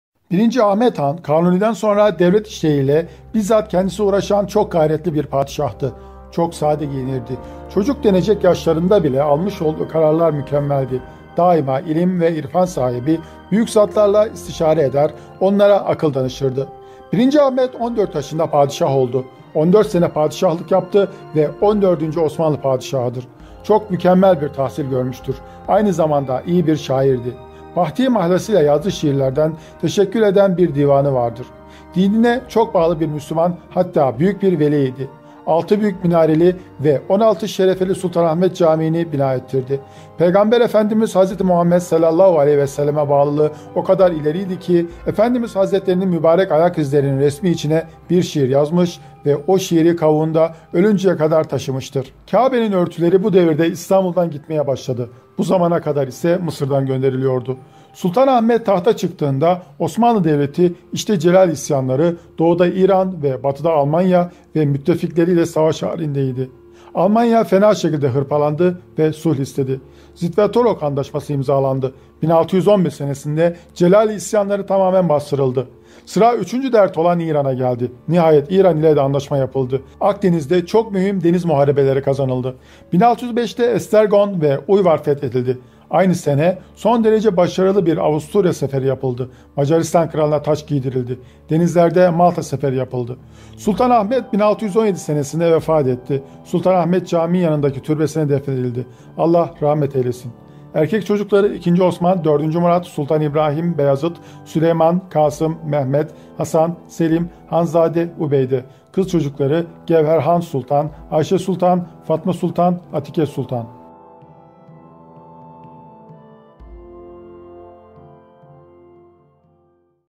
Osmanlı Padişahları'nın Hayatı (14) - 1. Ahmed Han HD
Babası: Üçüncü Mehmed
Annesi: Handan Sultan
Doğumu: 18 Nisan 1590
Vefatı: 22 Kasım 1617
Saltanatı: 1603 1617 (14 Sene)

Birinci Ahmed, Kanuni'den sonra devlet işleri ile bizzat kendisi uğraşan çok gayretli bir padişahtı. Çok sade giyinirdi. Çocuk denecek yaşlarında bile almış olduğu kararlar mükemmeldi. Daima ilim ve irfan sahibi büyük zatlarla istişare eder, onlara akıl danışırdı.

Birinci Ahmed, 14 yaşında padişah oldu. 14 sene padişahlık yaptı ve on dördüncü Osmanlı padişahıdır. Çok mükemmel bir tahsil görmüştür. Aynı zamanda iyi bir şairdi. Bahti mahlasıyla yazdığı şiirlerinden teşekkül eden bir divanı vardır.

Dinine çok bağlı bir Müslüman hatta büyük bir veli idi. Altı büyük minareli ve 16 şerefeli Sultanahmet Camiini bina ettirdi. Peygamberimiz Hz. Muhammed Efendimize (s.a.v.)bağlılığı o kadar ileri idi ki, Efendimiz Hazretlerinin mübarek ayak izlerinin resmi içine bir şiir yazmış ve o şiiri kavuğunda ölünceye kadar taşımıştır. dersimiz.com O şiir ise şudur:

N'ola tâcım gibi başımda götürsem dâim,
Kadem-i resmini ol Hazreti şâhı Rusülün.
Gül-i Gülzâri Nübüvvet, o kadem sahibidir.
Ahmedâ durma yüzün sür kademine ol gülün.

Kâbe'nin örtüleri bu devirde İstanbul'dan gitmeye başladı. Bu zamana kadar ise Mısır'dan gönderiliyordu.

Sultan Ahmed tahta çıktığında, Osmanlı Devleti, içte Celâli isyanları, doğuda İran ve batıda Almanya ve müttefikleri ile savaş halinde idi. Almanya fena şekilde hırpalandı ve sulh istedi. Zitvatorok Antlaşması imzalandı. 1611 senesinde Celâli isyanları tamamen bastırıldı. Sıra üçüncü dert olan İran'a geldi. Nihayet İran ile de antlaşma yapıldı. Akdeniz'de çok mühim deniz muharebeleri kazanıldı.

1605'de Estergon ve Uyvar fethedildi. Aynı sene son derece başarılı bir Avusturya seferi yapıldı. Macaristan Kralına taç giydirildi. Denizlerde Malta seferi yapıldı.

Sultan Ahmed 1617 senesinde vefat etti. Sultanahmed Camii yanındaki türbesine defnedildi. (Allah rahmet eylesin.)

Tesâniyi sahibi Mevlânâ Aliyyül Kaari (H.1014), Muğnillebib şârihi Şeyh Ebü Abdullah Muhammed (H. 1018), Hattat Hasan Çelebi Üsküdari (H. 1023) ve Karaca Ahmed (H. 1024)Sultan Ahmed devrinde vefat etmiş büyük zatlardır.

Erkek Çocukları: İkinci Osman, Dördüncü Murad, Sultan İbrahim, Bayezid, Süleyman, Kasım, Mehmed, Hasan, Selim, Hanzâde, Ubeyde.

Kız Çocukları: Gevherhan Sultan, Ayşe Sultan, Fatma Sultan, Atike Sultan.